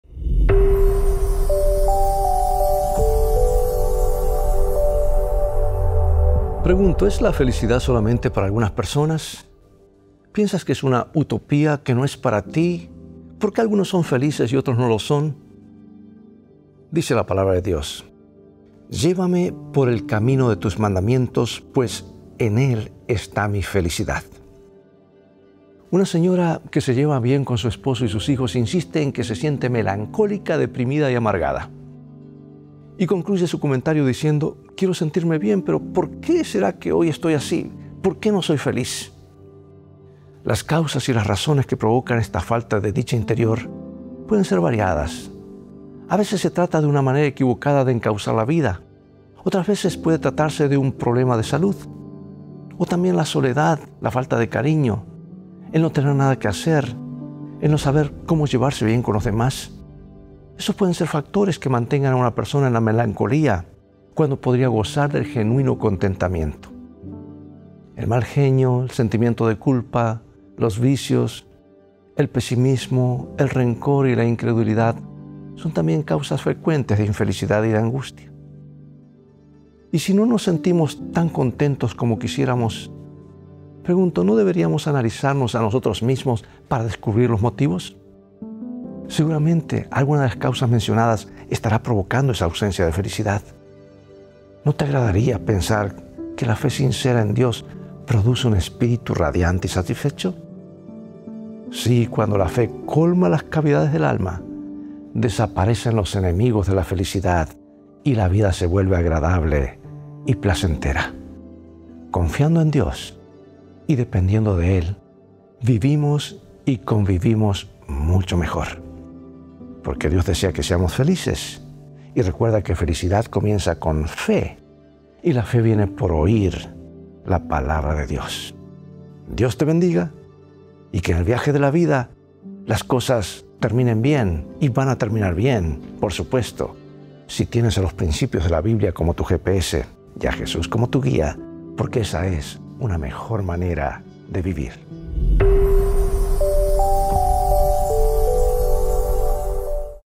Mejor Manera de Vivir - ¿Es la felicidad solo para algunos?
Cuando la fe colma las cavidades del alma, desaparecen los enemigos de la felicidad y la vida se vuelve agradable y placentera.